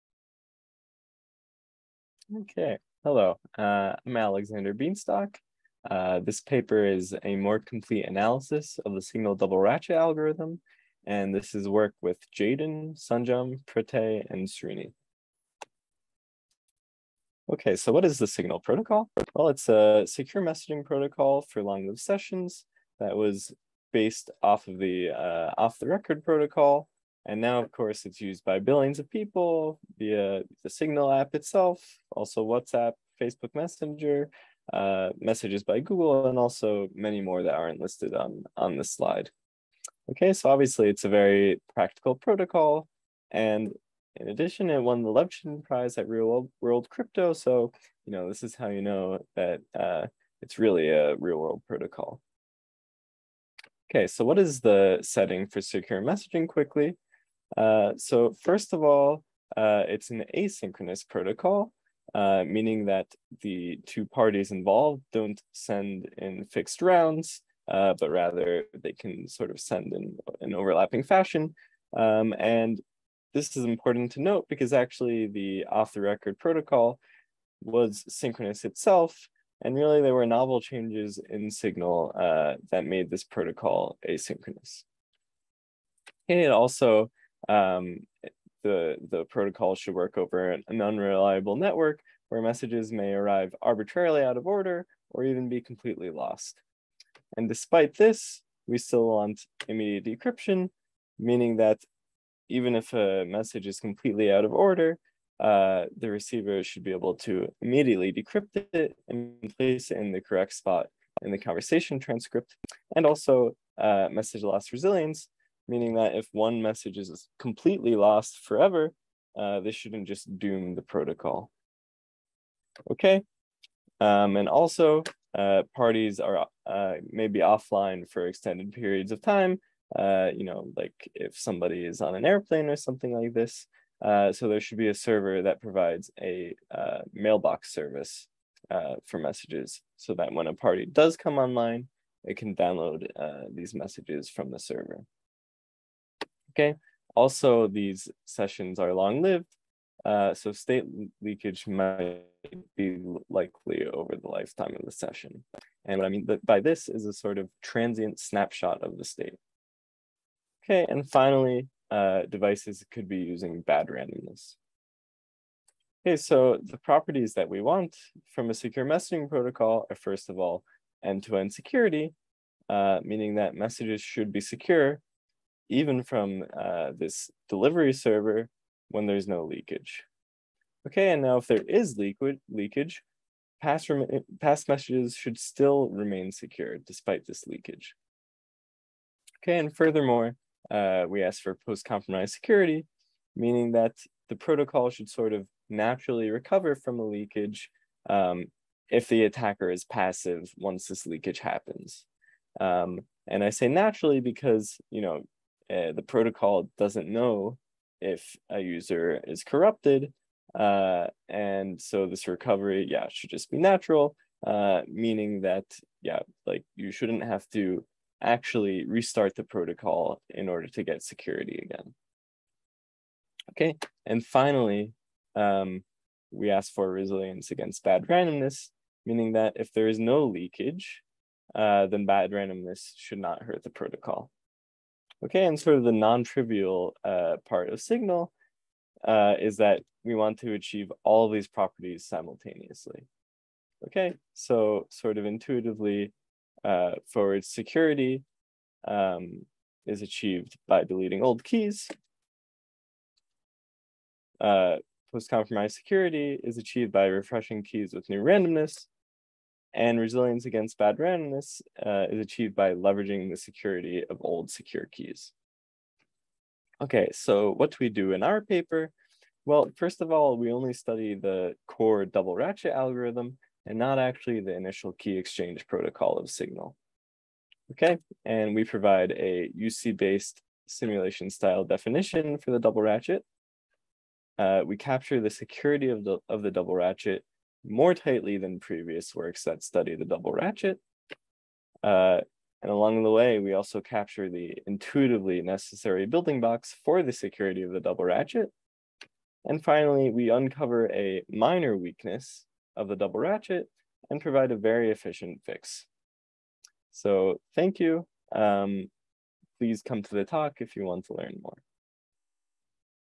A More Complete Analysis of the Signal Double Ratchet Algorithm
Paper by Alexander Bienstock, Jaiden Fairoze, Sanjam Garg, Pratyay Mukherjee, Srinivasan Raghuraman presented at Crypto 2022